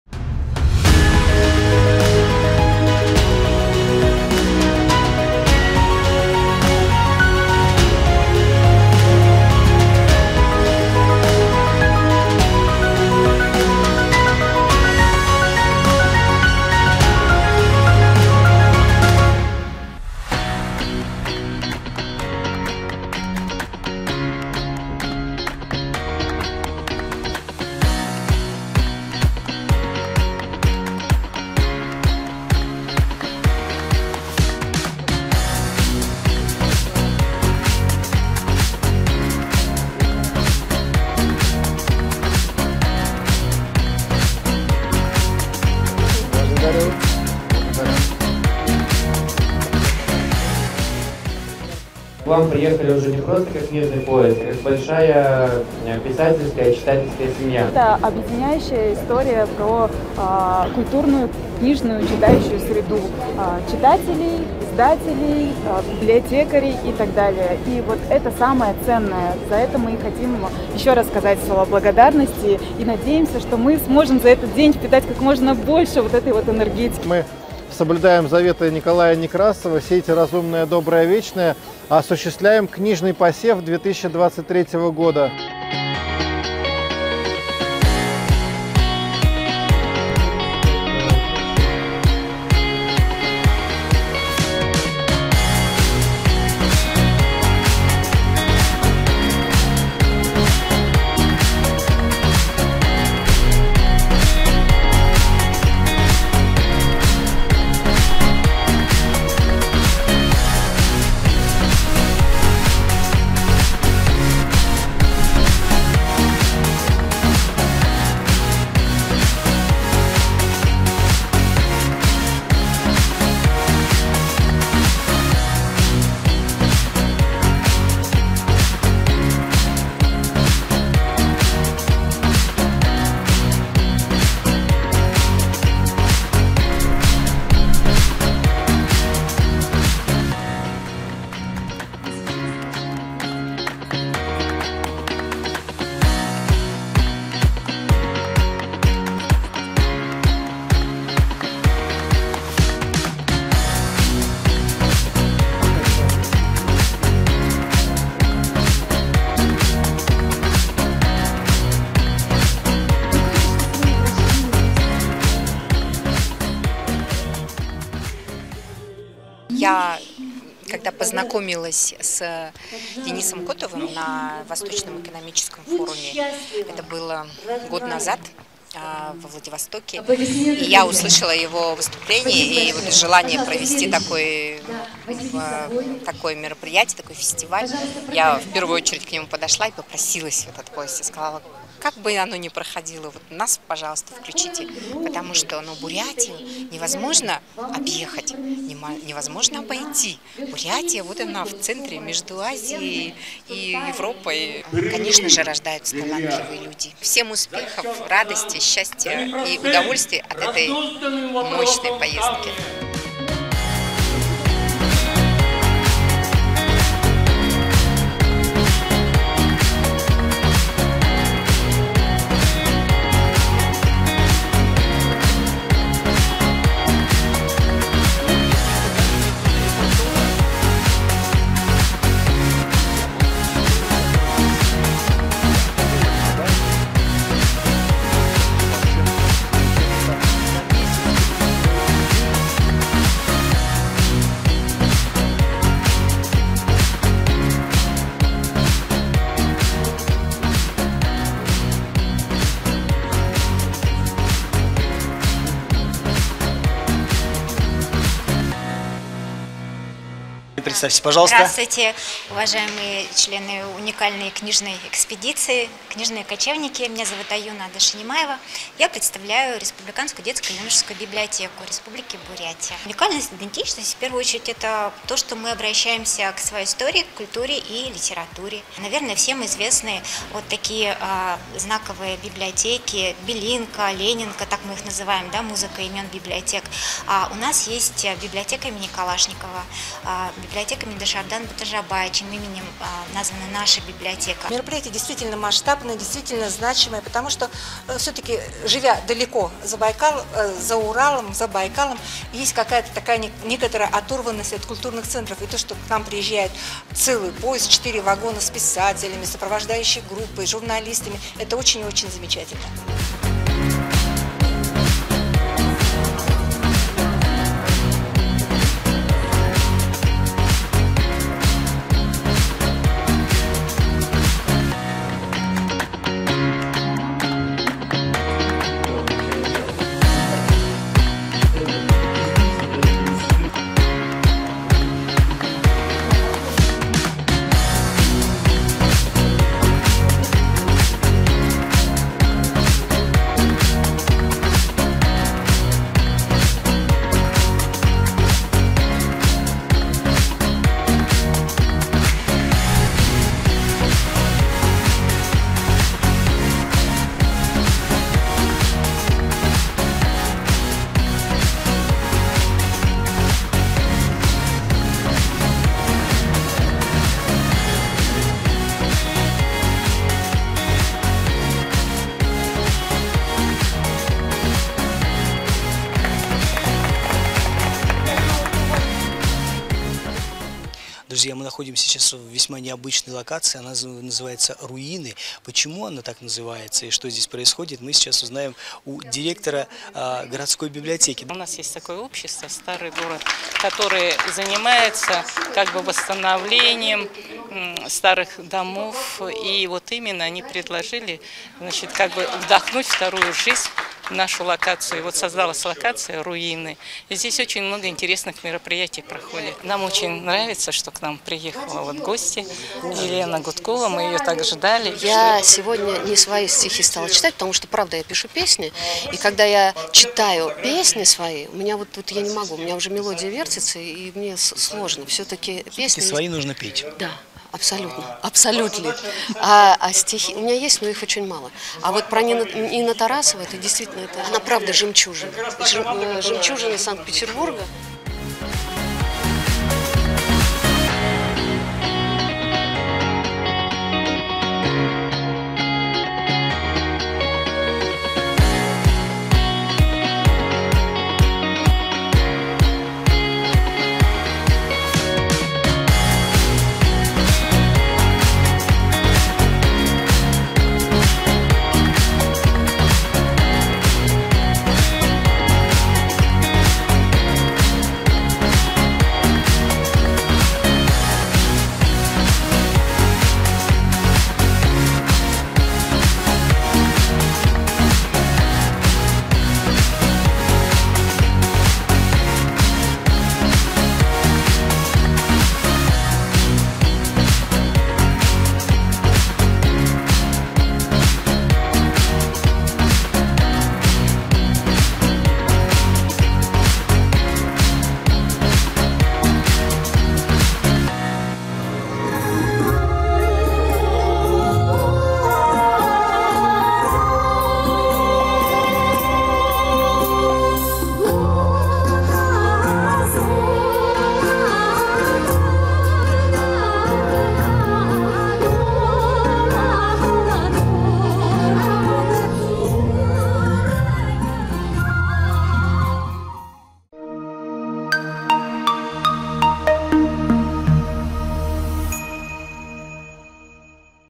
Книжная экспедиция. День двенадцатый. Улан-Удэ
Книжный поезд прибывает в солнечный, красивый и многонациональный город, где сплелись традиции и обычаи разных народов — Улан-Удэ. Улыбками и яркой самобытностью встречают путешественников радушные жители Бурятии, и с ответной благодарностью выступил директор книжного поезда проекта «Книжные маяки России» Максим Афоненков.

По пути к Национальной библиотеке участники Книжной экспедиции наслаждаются красотами города, тесно соседствующими с неповторимой природой края. А во внутреннем дворике их ожидает великолепный концерт и обмен книгами.

Перед началом пленарного заседания Министр культуры Республики Бурятия Соёлма Дагаева потрясающе интересно рассказывает о том, как пригласила Книжную экспедицию в свой гостеприимный край после выступления президента Фестивального движения «Книжные маяки России» Дениса Котова во Владивостоке.

Раритетные кадры с пленарного заседания и Круглого стола никого не оставят равнодушным. Это азарт и готовность к новым свершениям, доброжелательные дискуссии и интервью. Та же добрая атмосфера света и тепла сопровождает нас на прогулке по чудесным улицам Улан-Удэ к уникальному творческому пространству «Руины», где всех желающих ожидают открытия, которые необходимо увидеть собственными глазами!

Завершает хронику великолепного дня праздничный концерт «Только СЛОВО» на Театральной площади.

А наш Книжный поезд отправляется дальше благодаря поддержке генерального партнера: ОАО «Российские железные дороги».